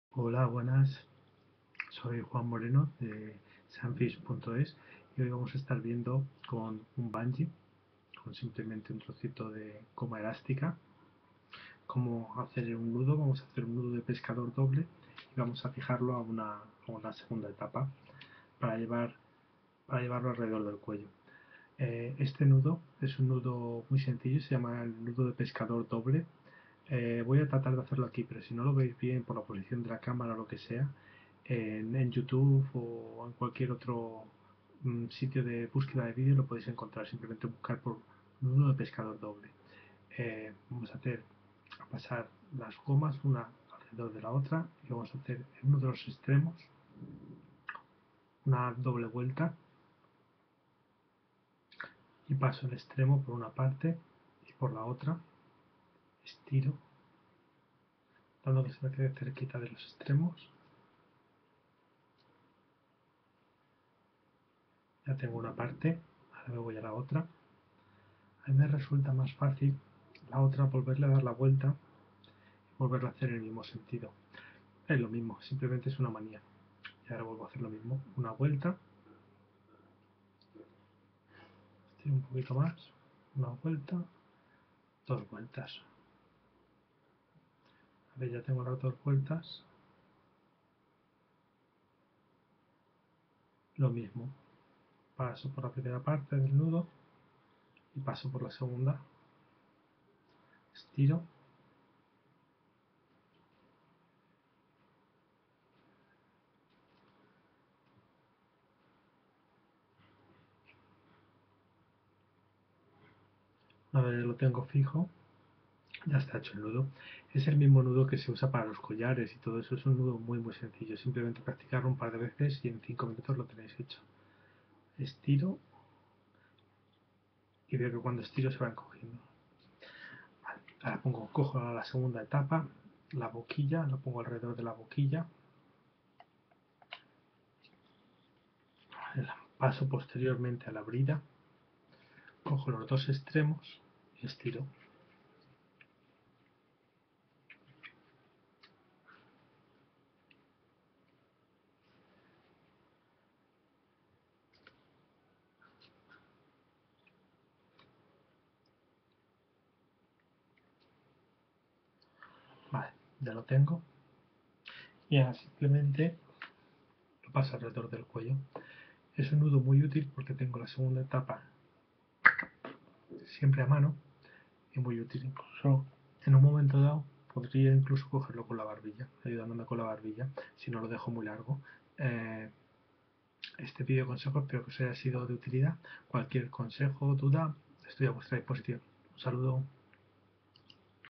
Nudo latiguillo corto tipo pescador doble
Uno de los nudos que se pueden usar para fijar el latiguillo corto de la segunda etapa del regulador alrededor del cuello